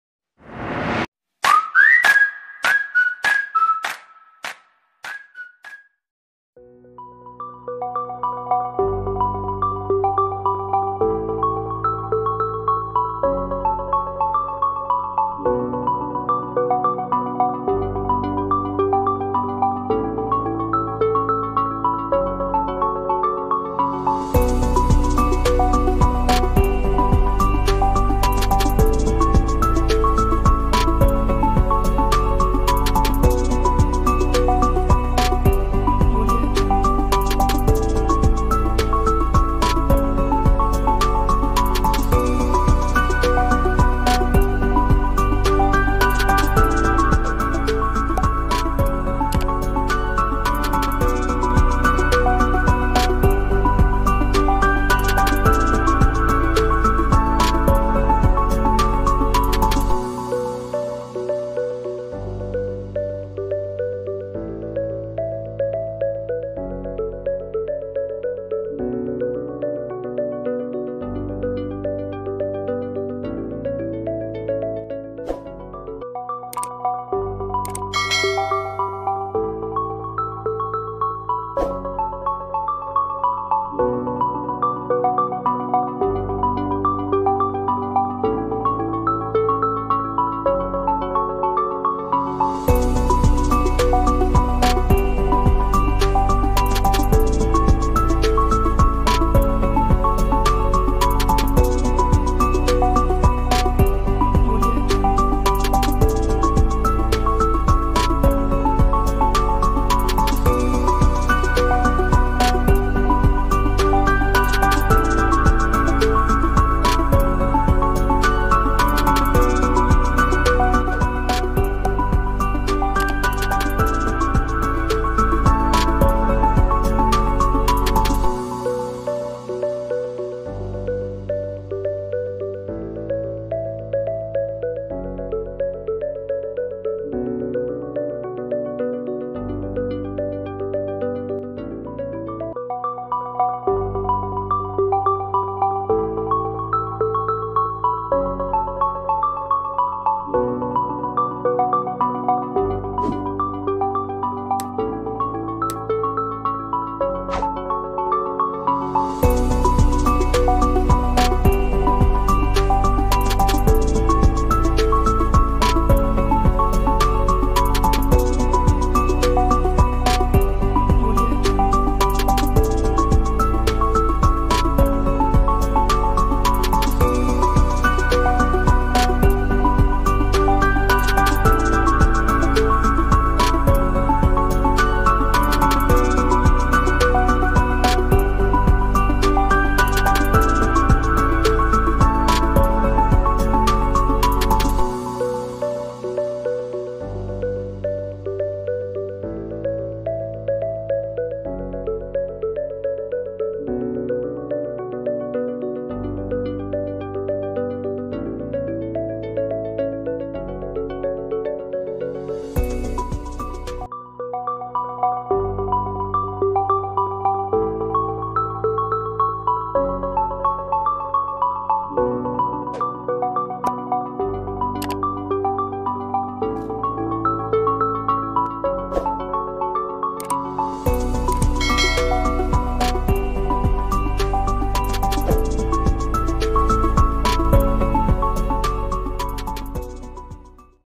How To Social Media Add On YouTube channel ।।Link Your Social Media YouTube channel ।।AllNewTips
হ্যালো বন্ধুরা আজকের ভিডিও থাকে তোমরা জানতে পারবে, কি করে তোমরা তোমাদের Youtube চ্যানেল আর সাথে Facebook,instagram ,& আরো সোশ্যাল মিডিয়া Add/যুক্ত করতে পারবে তো ফুল ভিডিও টা সবাই মনোযোগ দিয়ে দেখো 😊